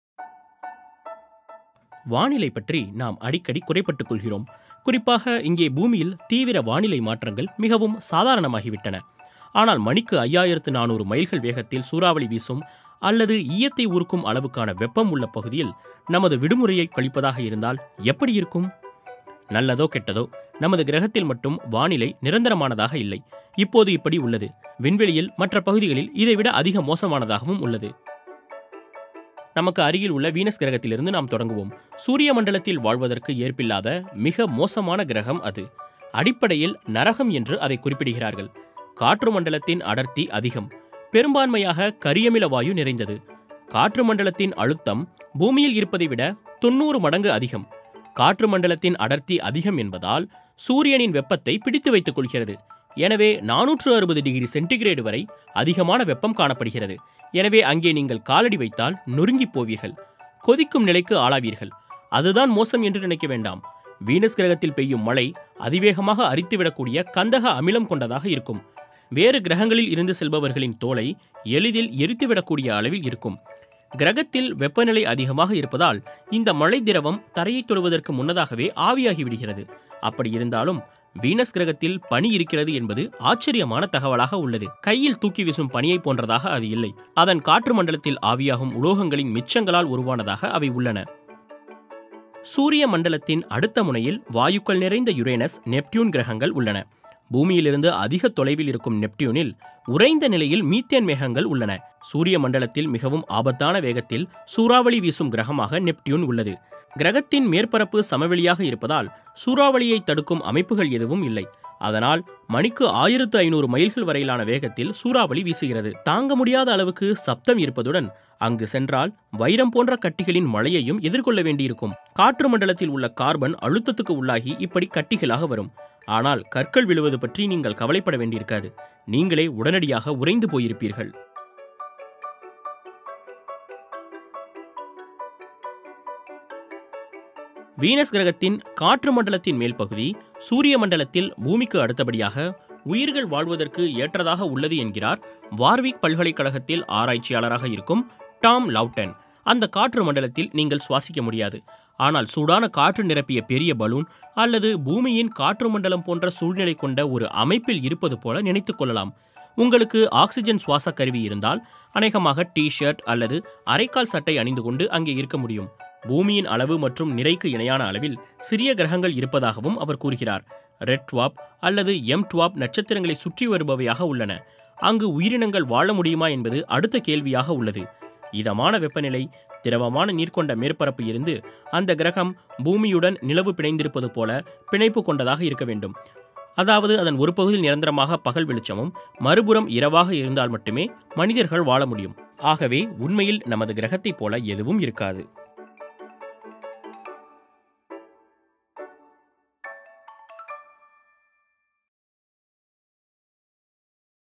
Weather on other planets - மனிதன் வாழ ஏற்ற காலநிலை எந்த கிரகத்தில் உள்ளது?
மழை, புயல், வெள்ளம், வெப்பம்: வேற்று கிரகங்களின் வானிலை எப்படி இருக்கும் தெரியுமா?

கந்தக அமில மழையிலிருந்து சூப்பர்சோனிக் மீத்தேன் காற்று வரையில், மற்ற கிரகங்களின் சூழ்நிலைகளைச் சமாளிக்க உங்களுக்குத் தண்ணீர் புகாத உடைகள் முதல் பல விஷயங்கள் தேவைப்படும்.

வானிலை பற்றி நாம் அடிக்கடி குறைபட்டுக் கொள்கிறோம். குறிப்பாக இங்கே பூமியில் தீவிர வானிலை மாற்றங்கள் மிகவும் சாதாரணமாகிவிட்டன. ஆனால் மணிக்கு 5,400 மைல்கள் வேகத்தில் சூறாவளி வீசும் அல்லது ஈயத்தை உருக்கும் அளவுக்கான வெப்பம் உள்ள பகுதியில் நமது விடுமுறையைக் கழிப்பதாக இருந்தால் எப்படி இருக்கும்?

நல்லதோ கெட்டதோ, நமது கிரகத்தில் மட்டும் வானிலை நிரந்தரமானதாக இல்லை - இப்போது இப்படி உள்ளது, விண்வெளியில் மற்ற பகுதிகளில் இதைவிட அதிக மோசமானதாகவும் உள்ளது.